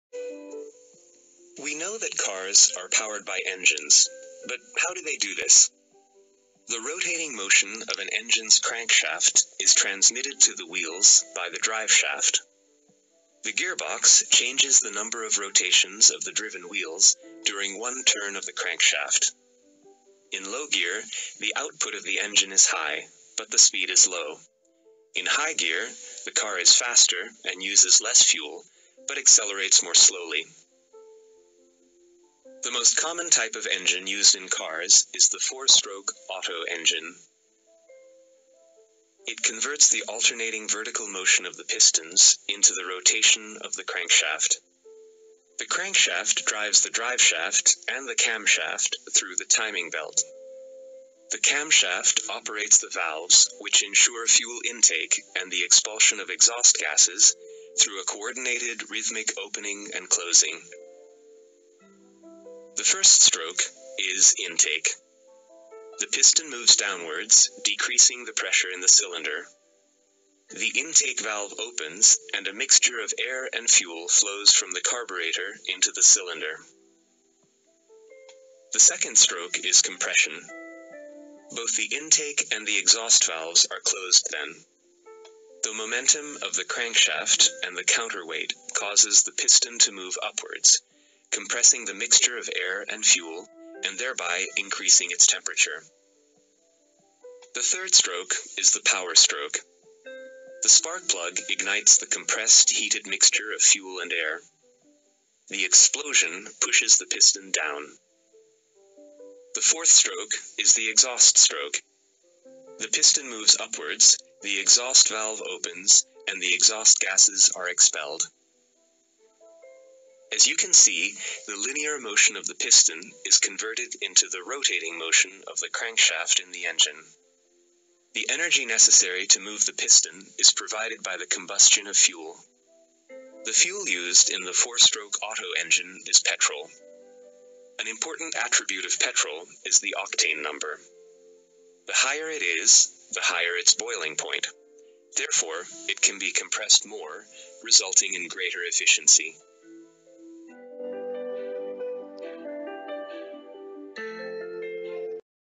How does a Four Stroke Engine works [© Hasrama Joni PG]
In this video i'm going to explain how a four-stroke engine works.
Learn more  about the basic components and the working of an four stroke automobile Diesel engine by watching this video.
Timestamps
0:00 Introduction and how a machine works
0:37 Engine and how it works
1:12 The First Phase (Aspiration)
1:31 The Second Phase (Compression)
1:54 The Third Phase (Expansion)
2:10 The Fourth Phase (Discharge)
2:23 The Relationship of Piston and Motor Axis Movement
2:35 THE Fuel